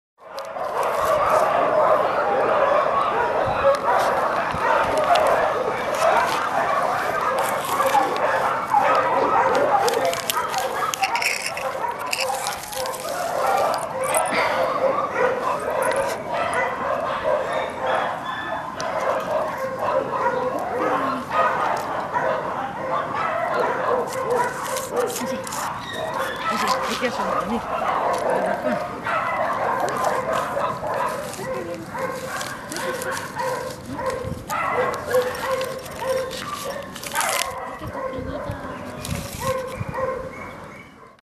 Gatito intentando coger salchichas
Este minino intentaba quitarnos un paquete de salchichas que usábamos para ayudar a una perrita que estaba asustada. En un descuido intentó sacar alguna que finalmente consiguió.